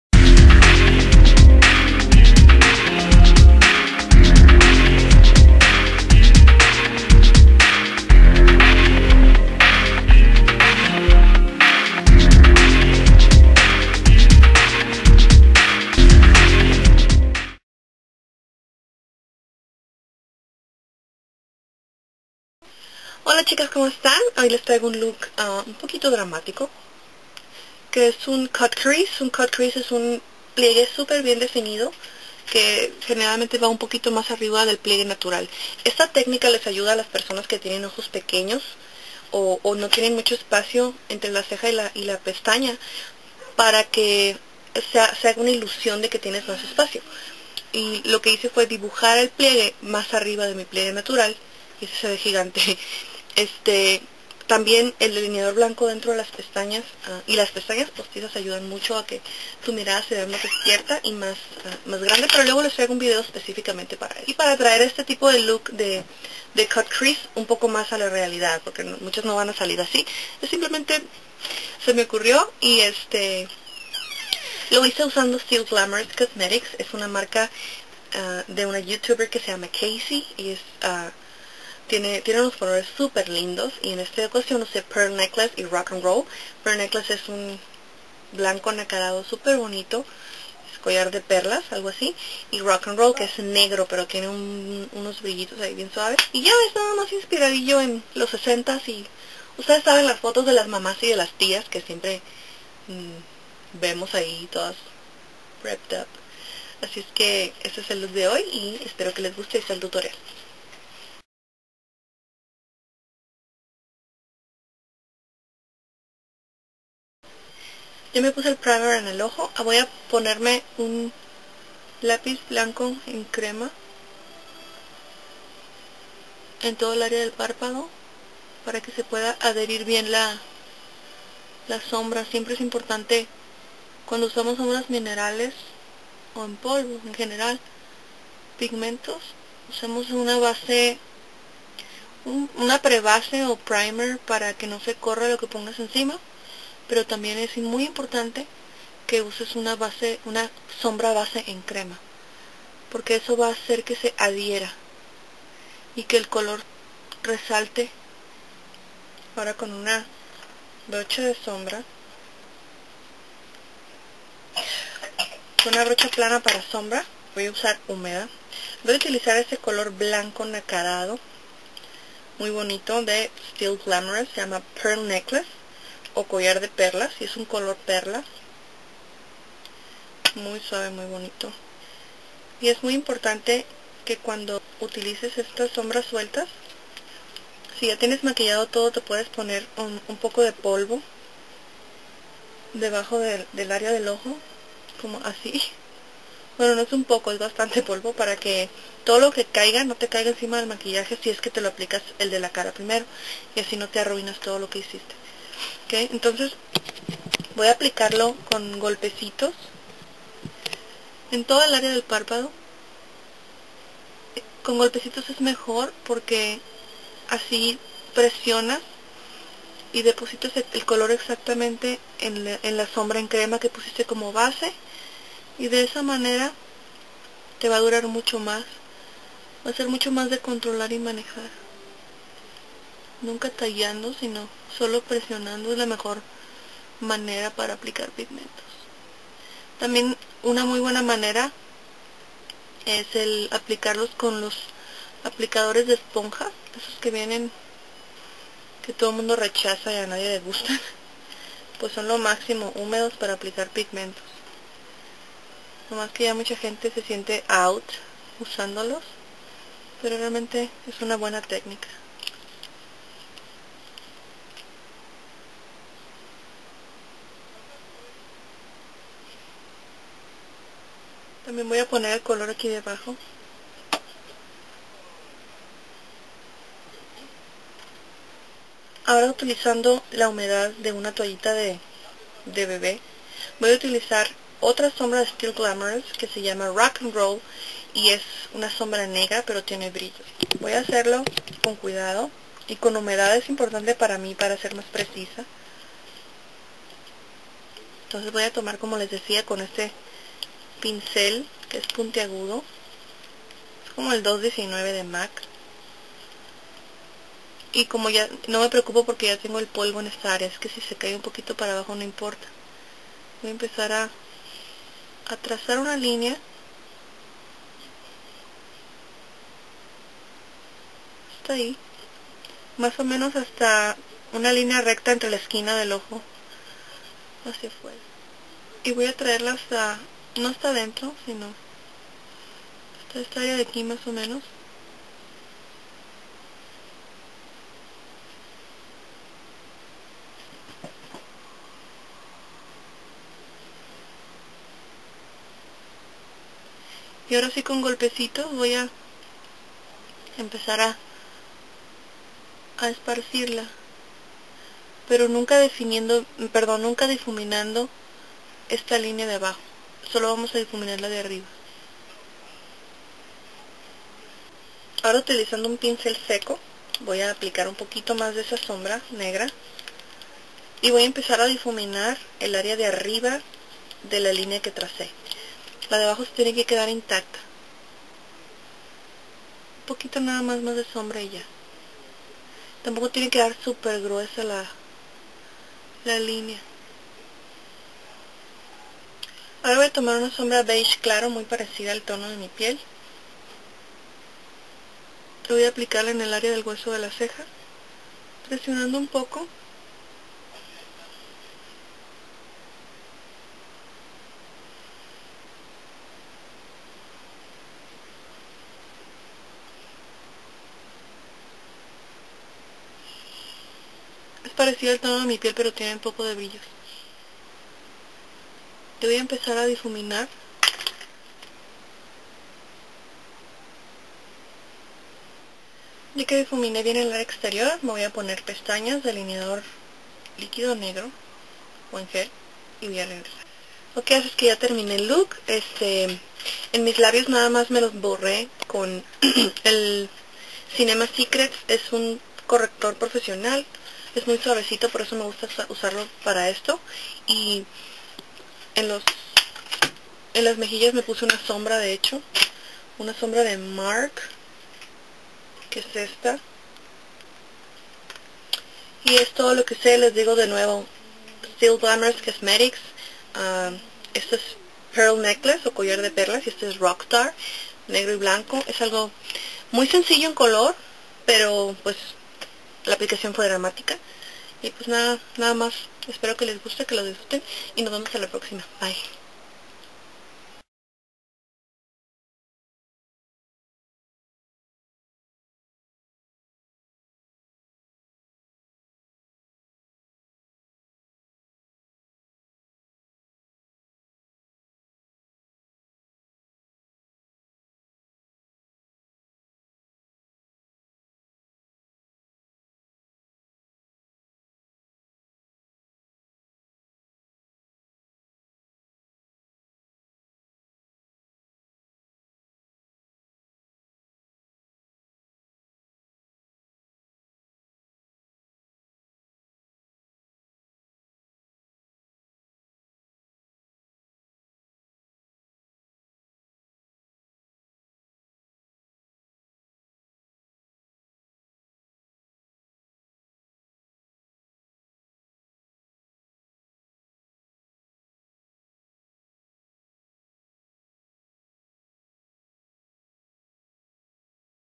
Tipo MOD...Still Glamorus
Chequen el sitio!
Still Glamorus Cosmetics
Maquillaje tipo mod, negro, blanco estilo 60's
Musica cortesia de: